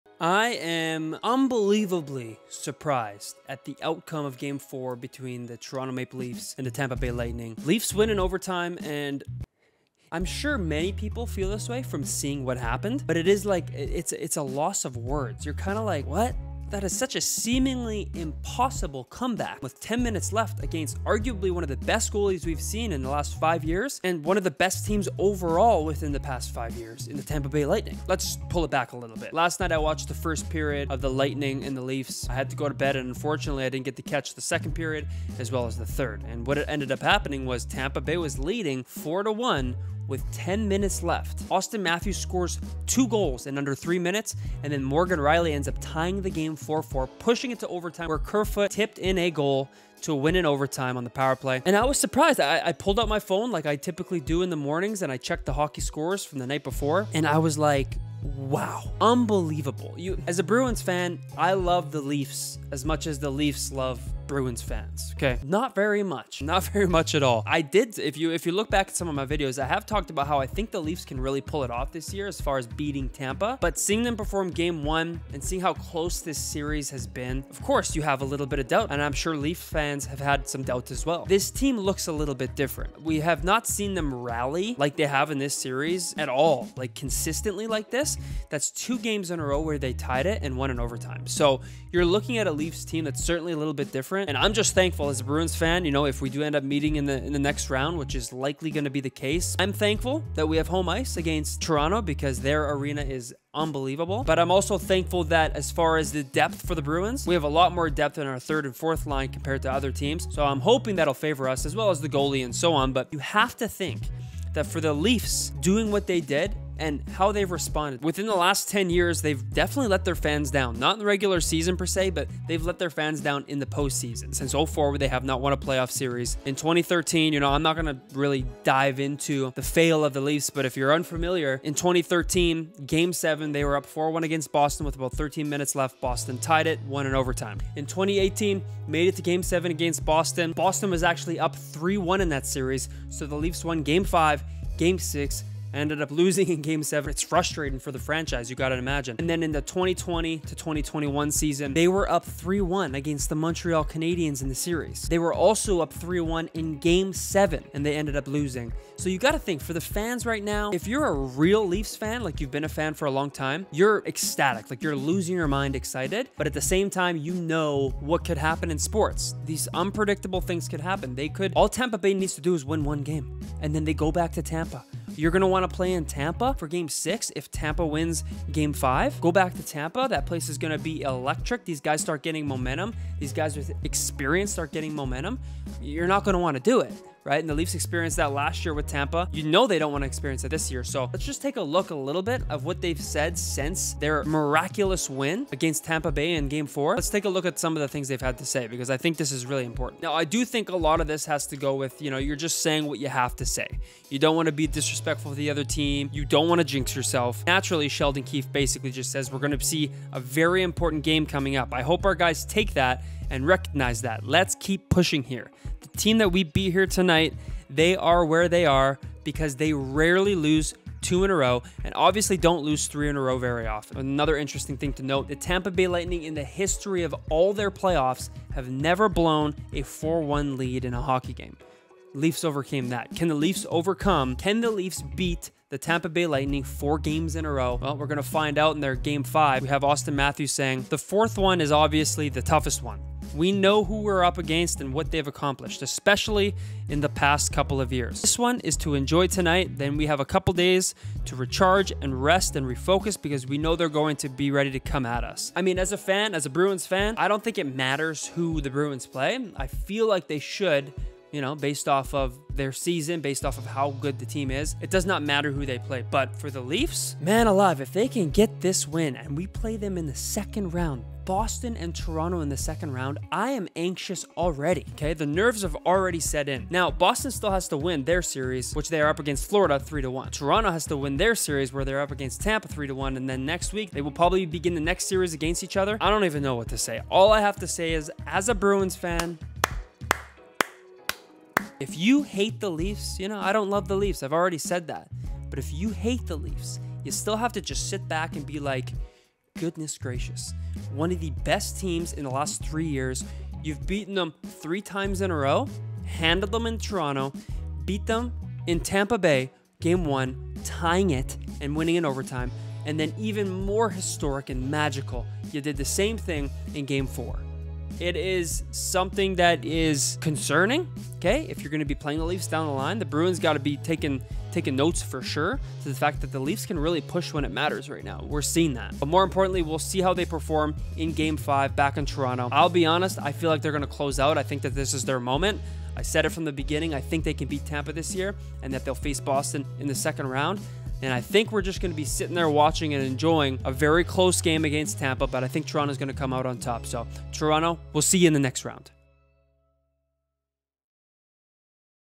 BRUINS FAN Talks LEAFS COMEBACK..
The Toronto Maple Leafs Completed an absolutely historic comeback last night and WOW..
We should probably discuss it.

Will we see another Bruins and Leafs playoff series?!